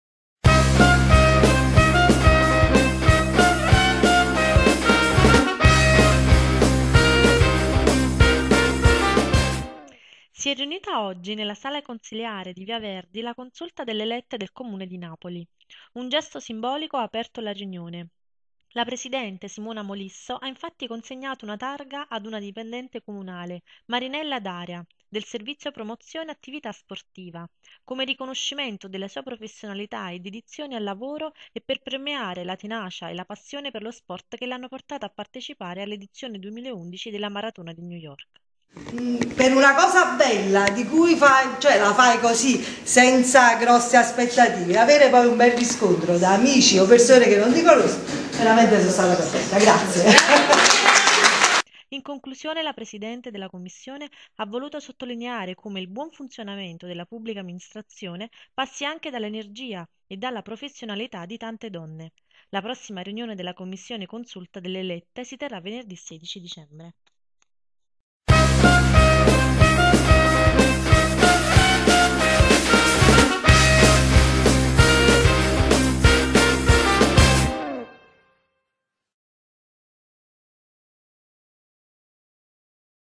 La Consulta delle Elette premia la maratoneta comunale
Si è riunita oggi, nella sala consiliare di via verdi la Consulta delle Elette del Comune di Napoli. Un gesto simbolico ha aperto la riunione, la presidente, Simona Molisso,  ha  infatti consegnato  una targa ad una dipendente comunale, Marinella D'Aria, del servizio Promozione Attività Sportiva,  come riconoscimento della sua professionalità e dedizione al lavoro, e  per premiare la tenacia e la passione per lo sport che l'hanno portata a partecipare all'edizione 2011 della maratona di New York. In conclusione la Presidente della Commissione ha voluto sottolineare come il buon funzionamento della pubblica amministrazione passi anche dall'energia e dalla professionalità di tante donne. La prossima riunione della Commissione Consulta delle Elette si terrà venerdì 16 dicembre.